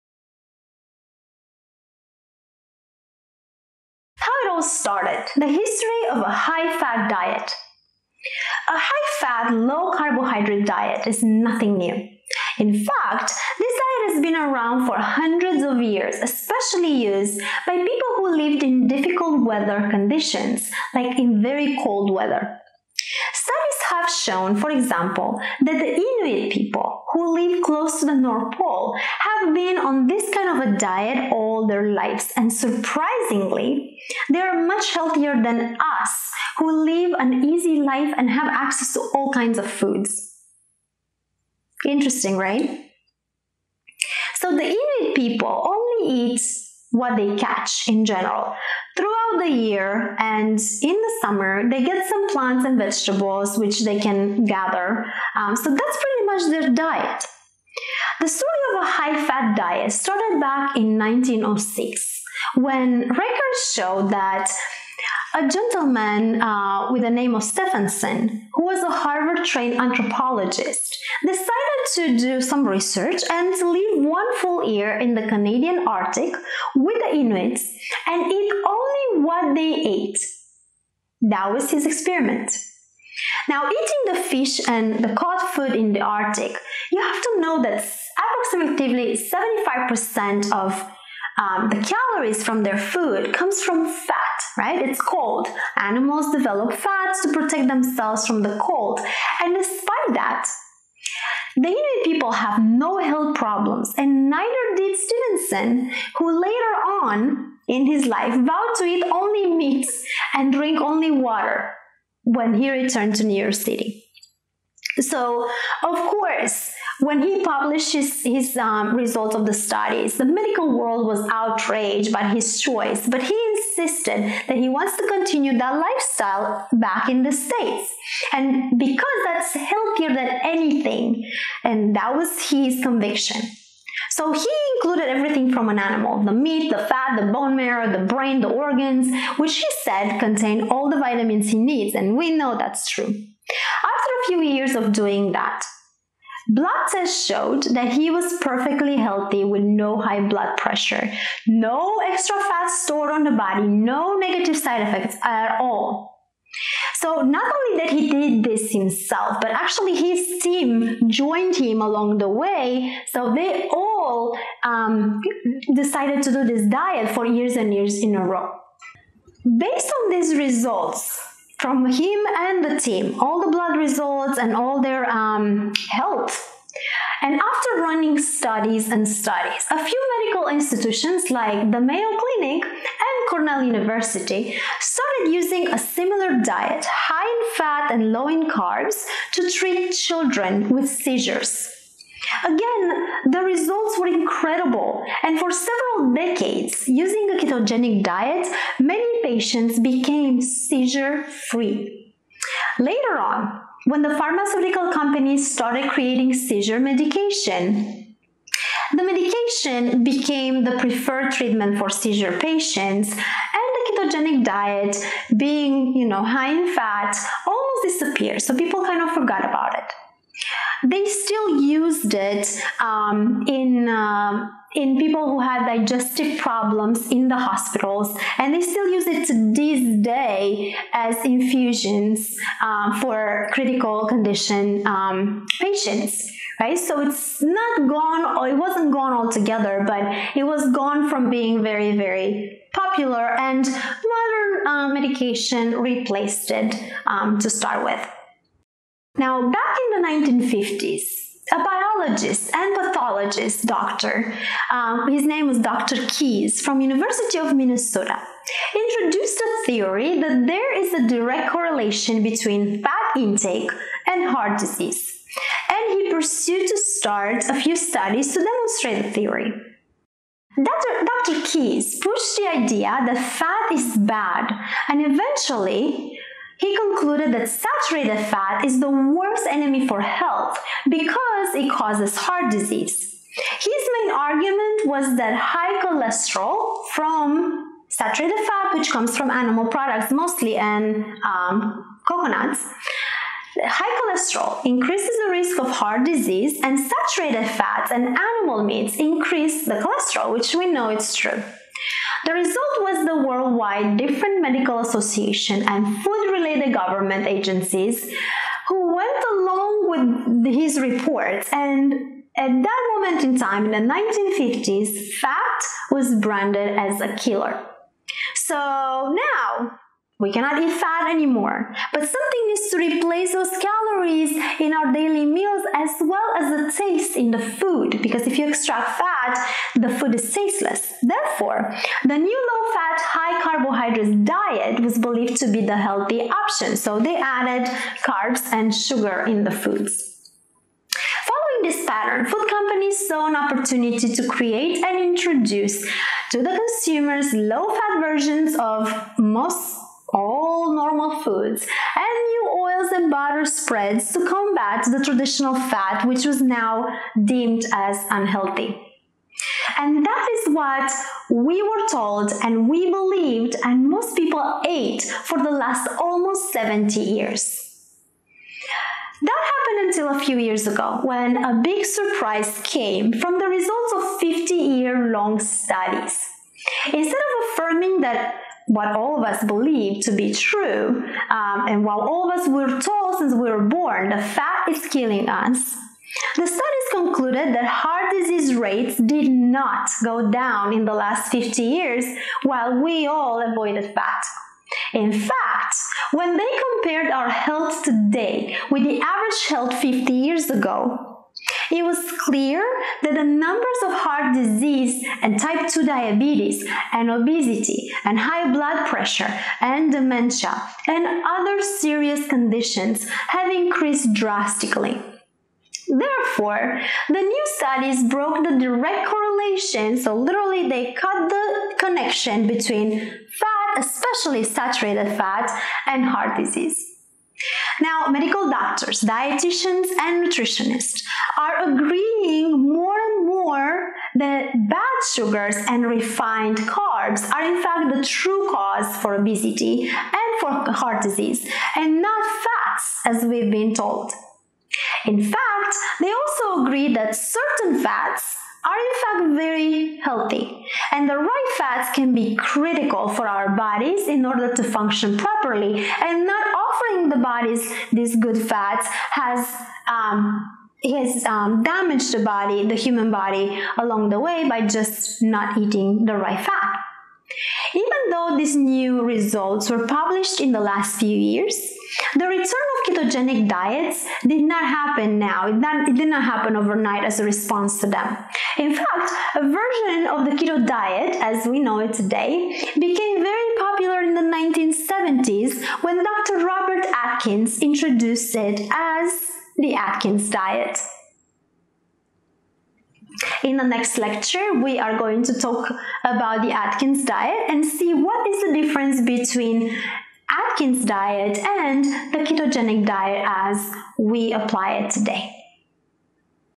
History of a High Fat Diet
The Ketogenic Diet is very trendy right now and everybody is talking about it, but is it really something new?
Watch this video and find out.. also, for the whole Ketogenic Diet and Ketosis Nutrition Health Coach Certification course please follow the